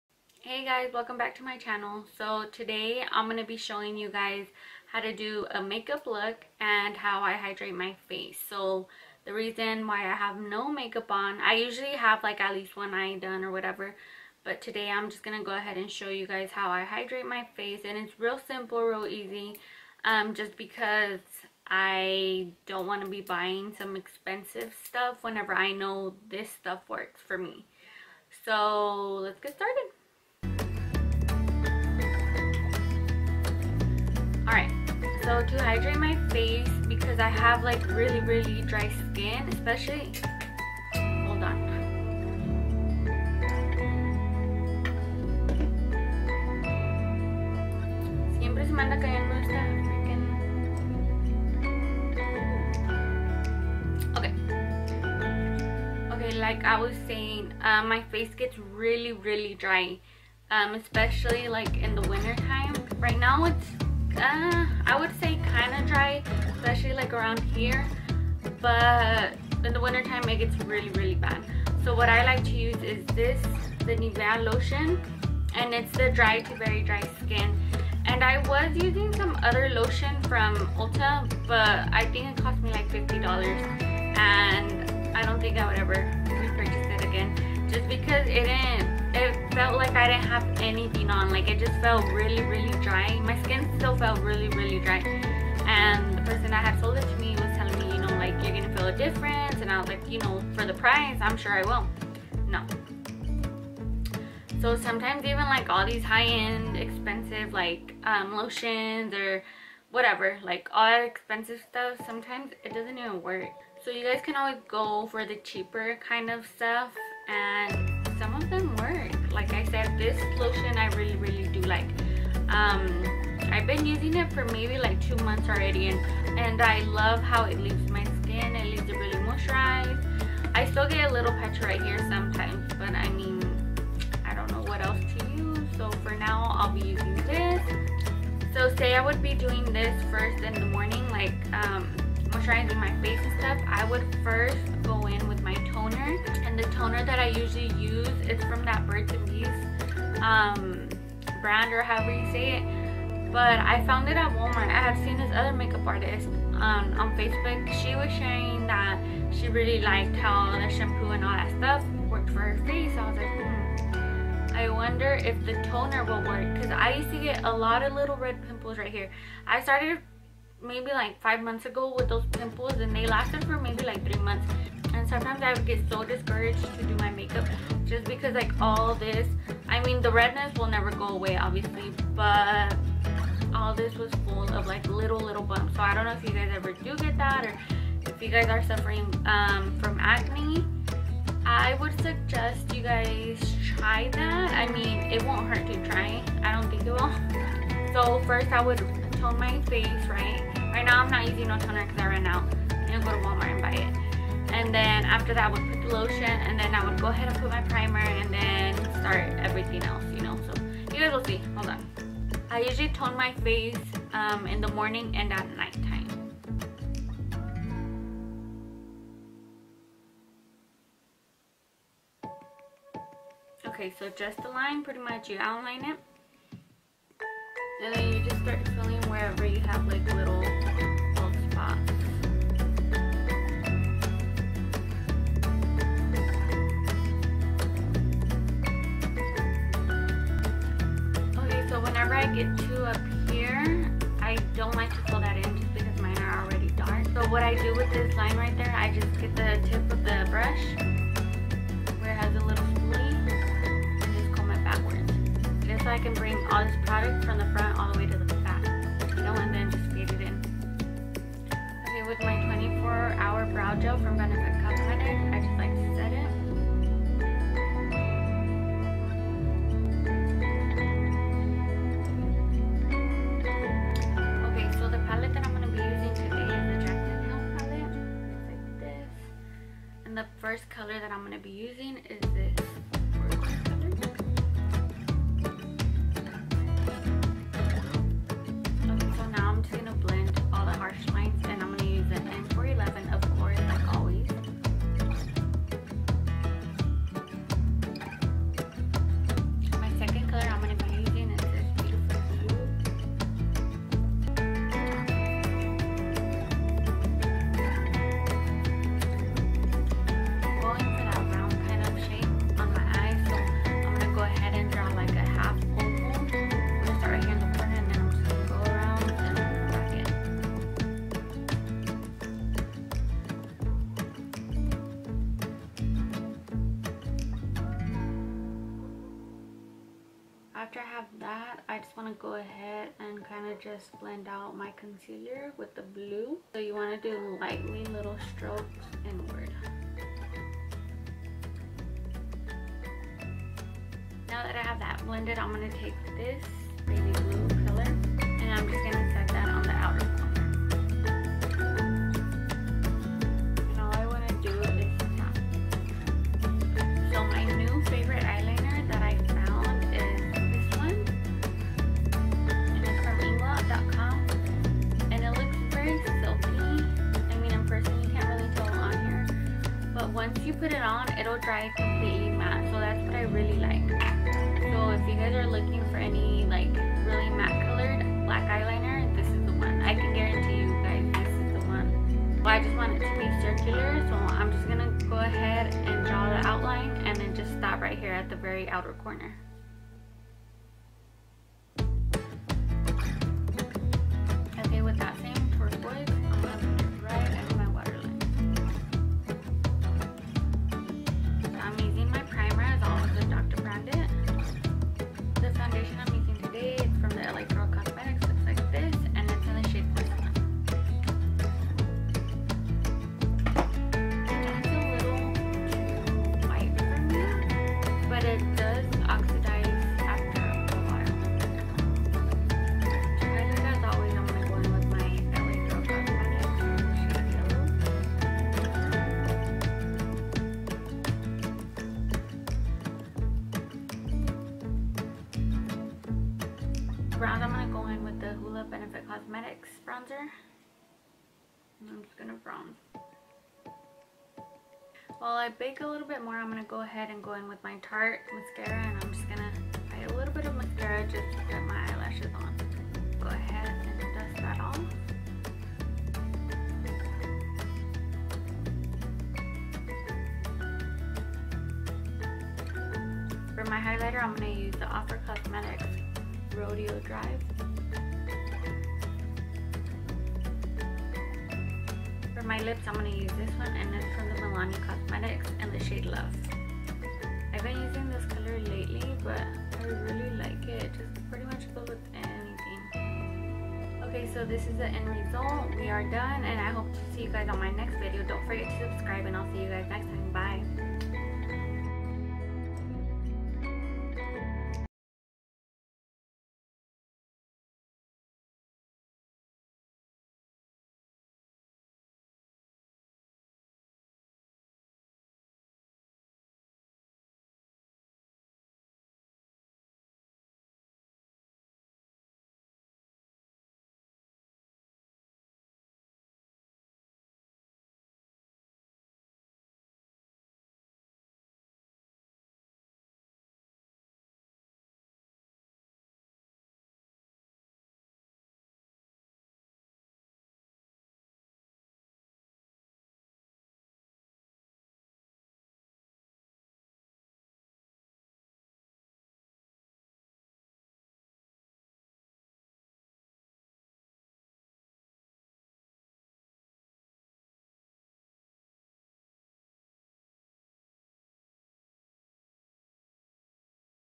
Quick and Easy Moisturize/deep blue cutcrease
Come See how I Moisturize real quick and do this beautiful Cutcrease!

Products Used
Brows: Benefit Cosmetics precisely pencil 24 hour brow gel

Face:
lagirl Porcelain. Its to light I would go darker
primer Dr Brandt
Bronzer Benefit cosmetics hoola bronzer
highlighter Ofra Cosmetics Rodeo Drive

Lips Milani Cosmetics in Love

SC:Rubeslove-21